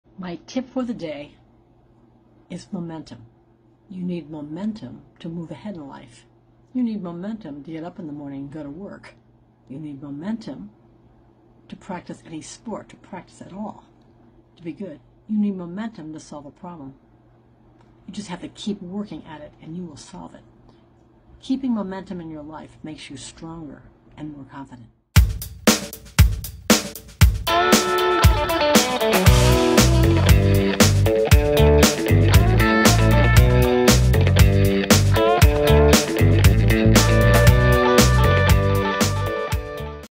ALI'S TUESDAY TIP-Jane Shellhouse
This video shows great tip from Jane in which she tells us to think about momentum. This is so important because we can stop and think before we speak. Valuable information.